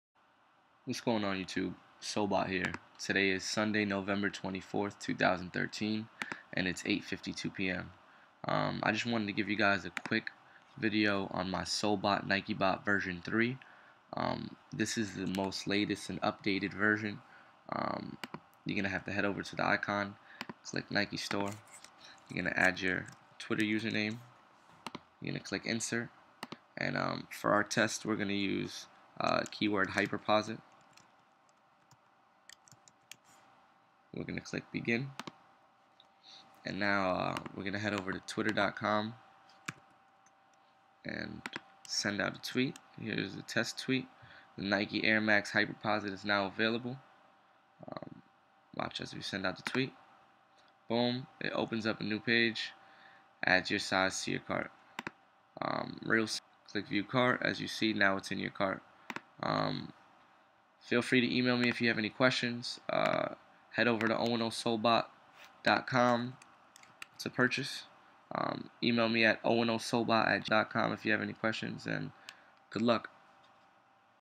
Nike.com Twitter Bot (V3 UPDATE)
This BOT will automatically open the link nike.com tweets, & proceeds to add your specified size to your cart.
This bot works on
Twitter.com & Nike.com